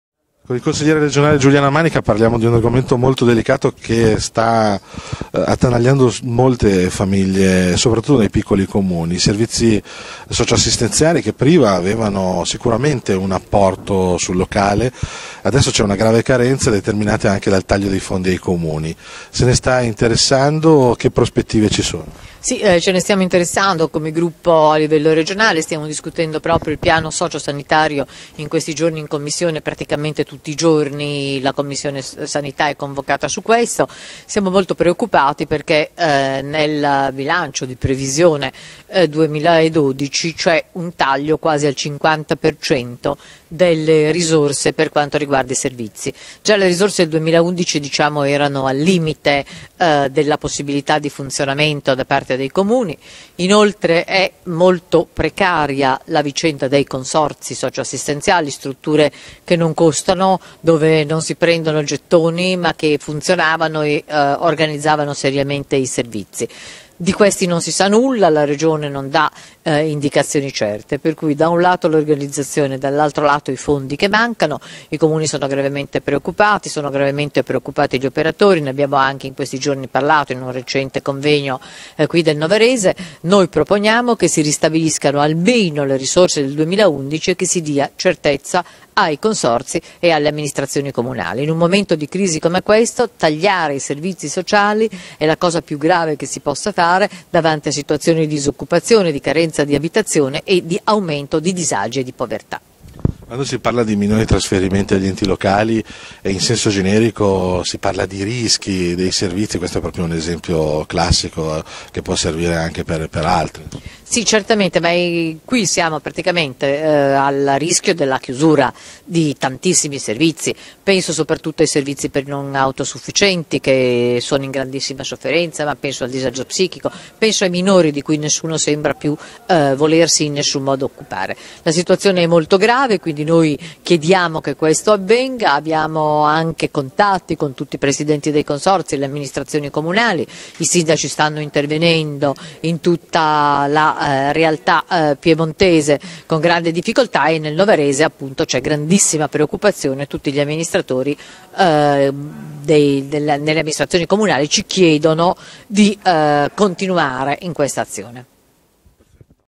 Socio Assistenziale_27.02.mp4
Giuliana Manica Consigliere Regionale del Partito Democratico a proposito delle iniziative a sostegno delle politiche socio assistenziali locali